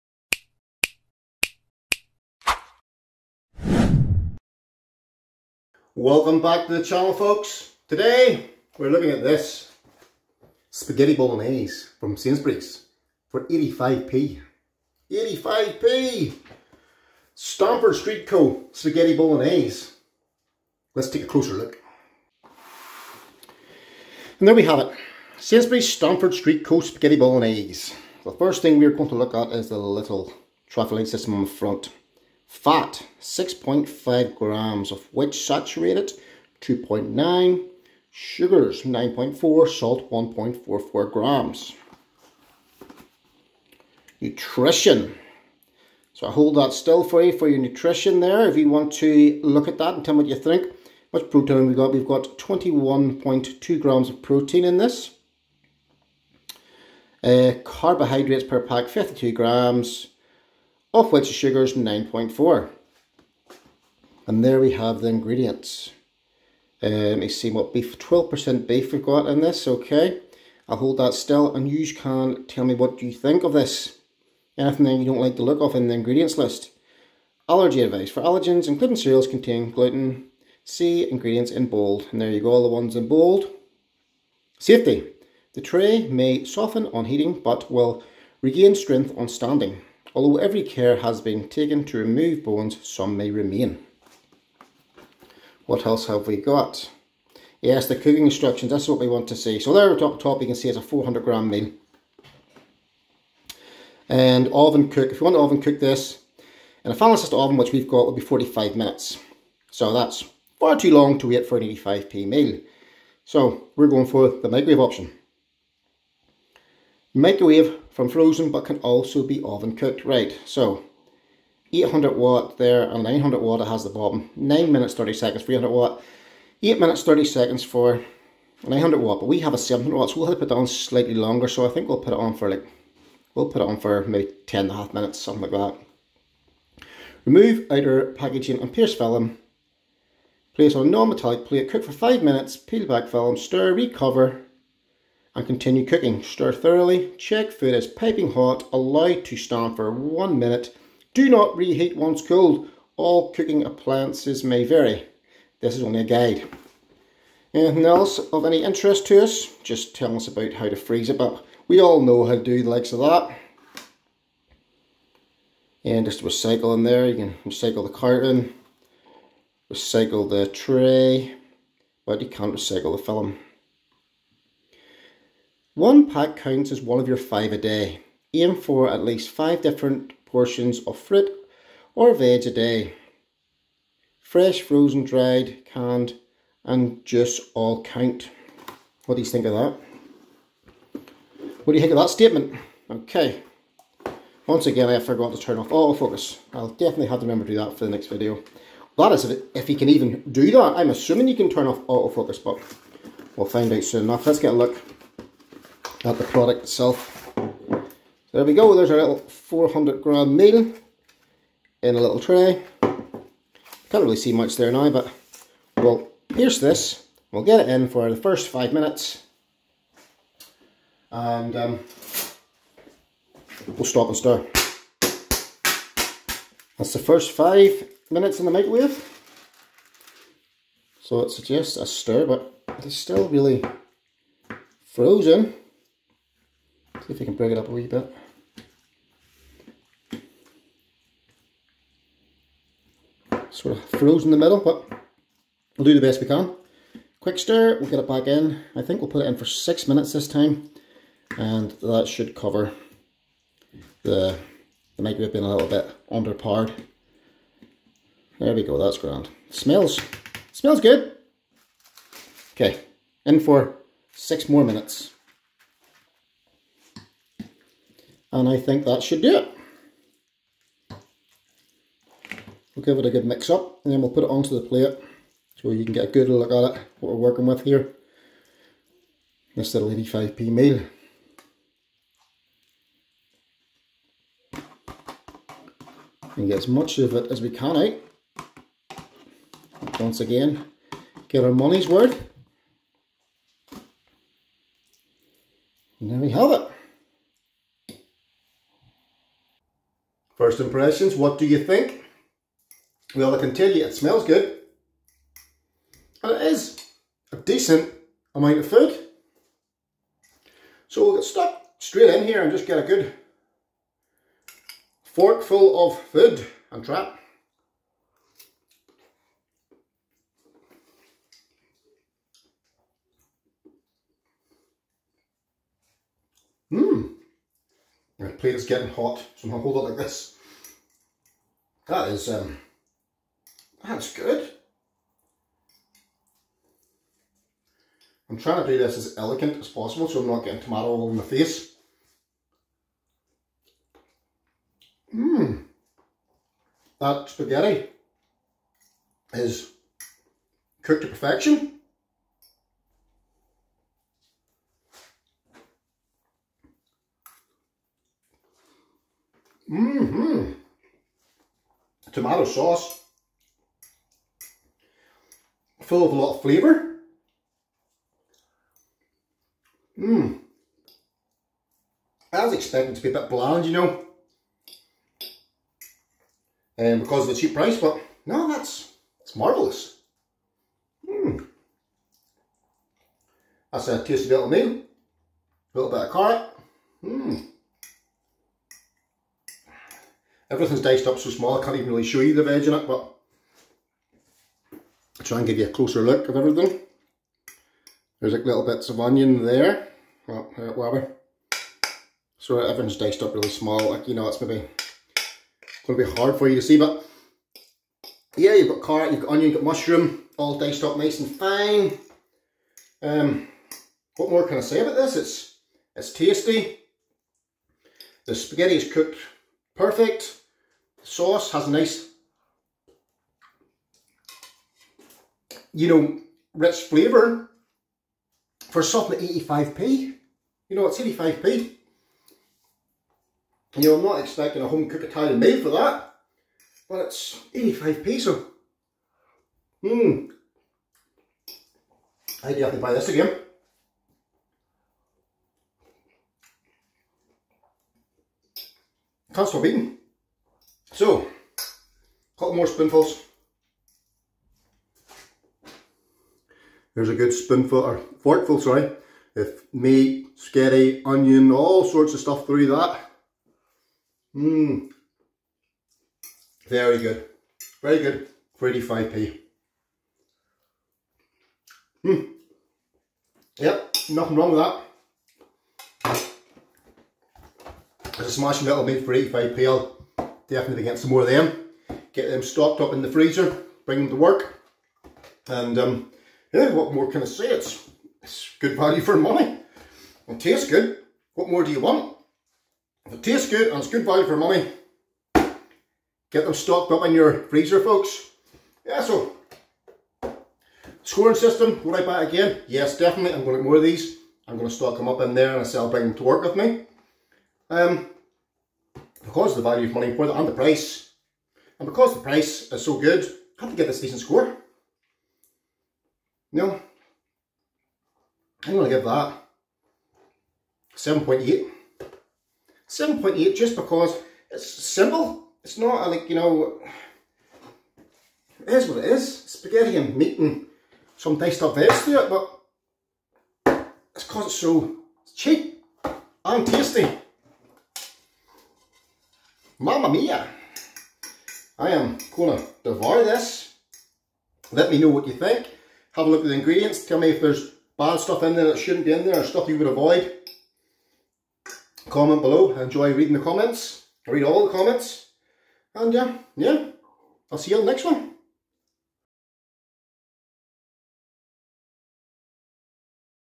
THIS IS CRAZY Spaghetti Bolognese by Sainsbury's
Sainsbury's have done it again 👏 £0.85 spaghetti bolognese. Great value for money.